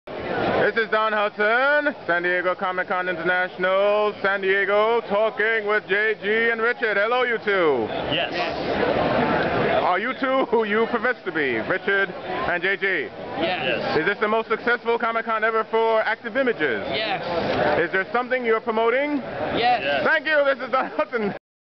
Richard Starkings and JG At Comic Con 2008
A short interview with the two creative kings of Active Images and Comicraft!